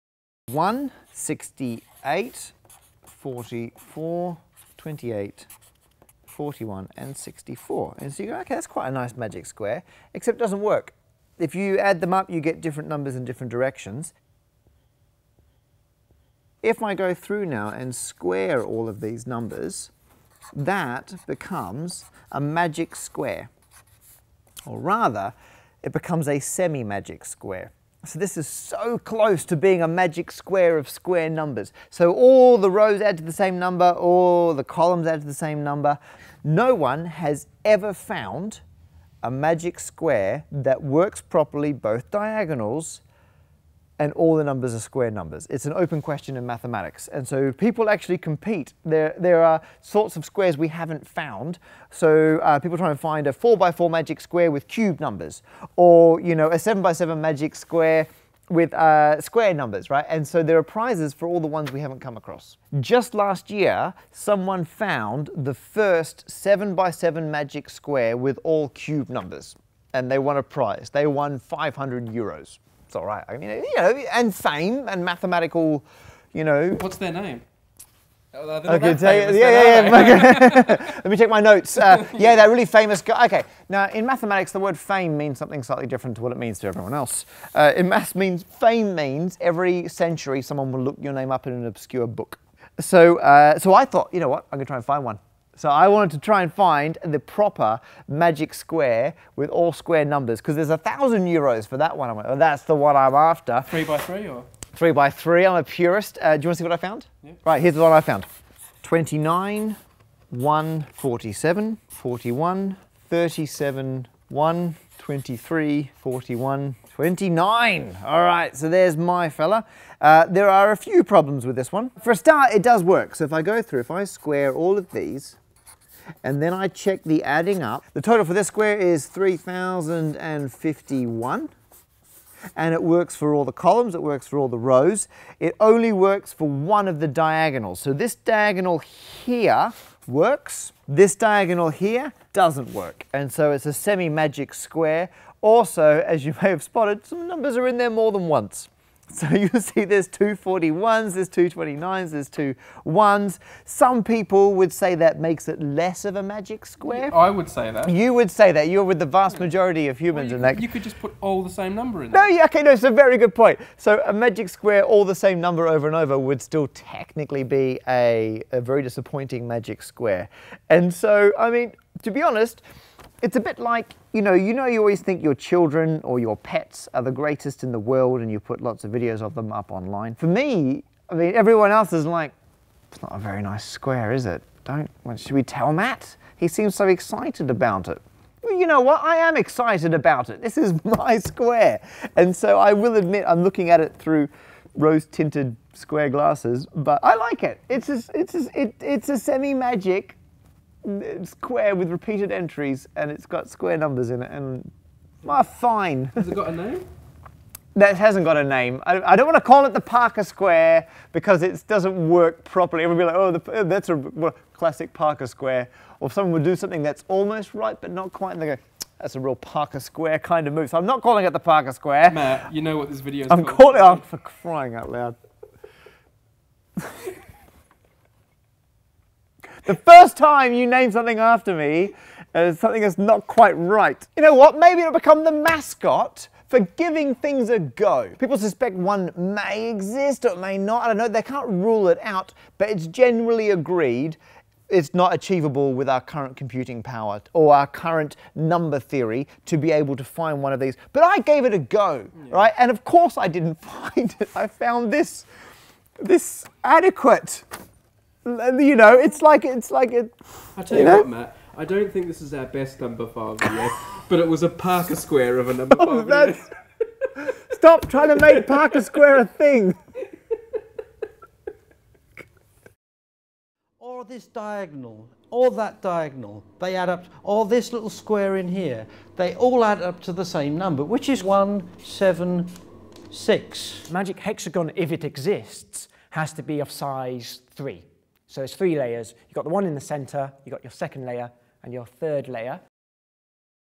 The Parker Square - Numberphile
Matt Parker reveals his pride and joy - the mysterious Parker Square! More links & stuff in full description below ↓↓↓